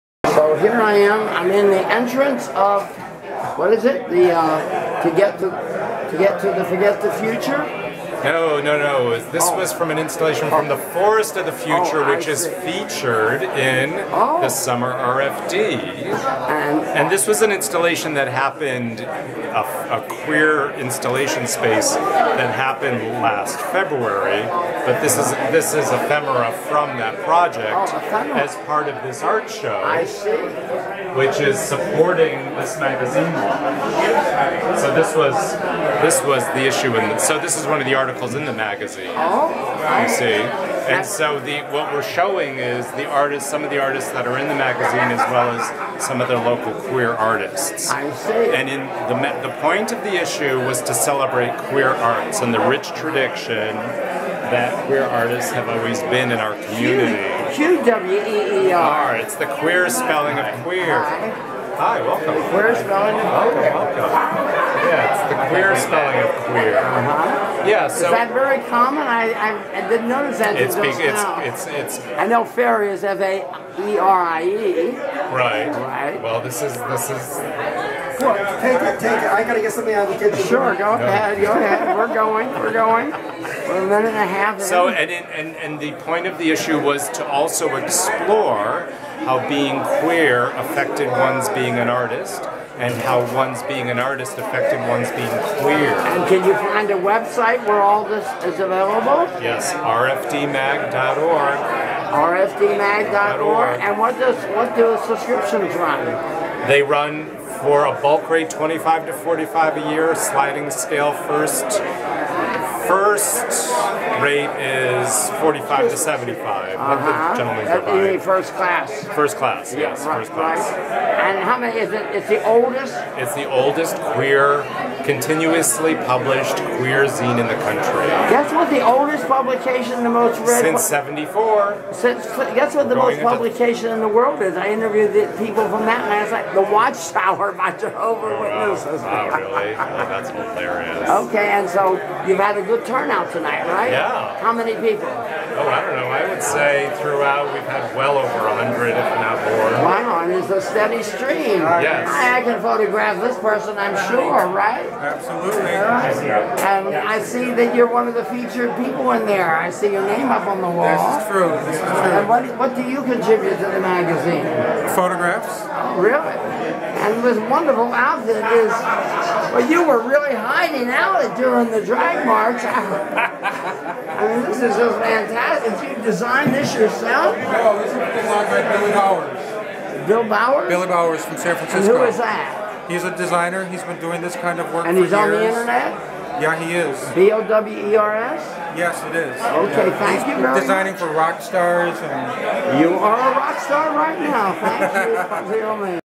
RFD MAGAZINE - QUEER ART ISSUE
RFD is the oldest continuously published LGBT publication in the USA.  It originally focused on rural living & gay folk.  It still focuses on those values combined with coverage of Radical Faerie Sanctuaries scatter throughout the world--a group founded by Harry Hay in 1979 embracing gender freedom in its many incarnations.